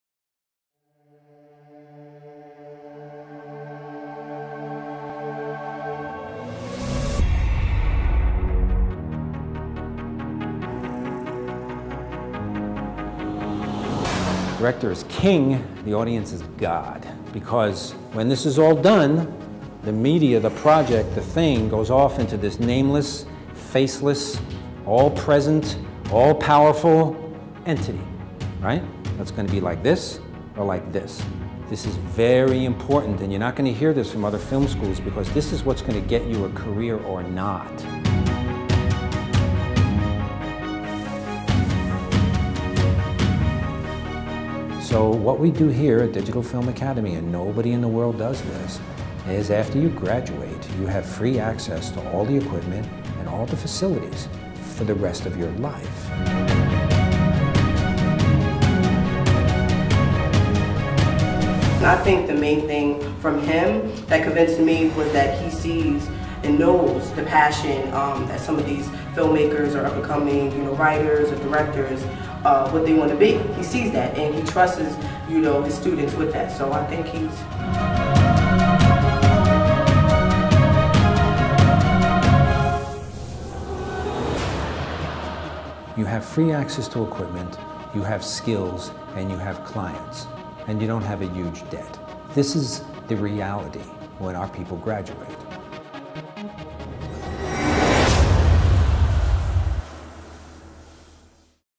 OPEN HOUSE at DIGITAL FILM ACADEMY, NEW YORK.
The OPEN HOUSE at DFA, New York is where you are taken behind the scenes to see what really happens at the school. Check it out to see why our students and Alumni are so successful in the era of digital media.

Patrick DiRenna discusses the LEAP program, Lifetime Access to Equipment and Facilities, and how we are the only film school that does that.

We invite you to sign up to observe a free class, or to attend our next Online Open House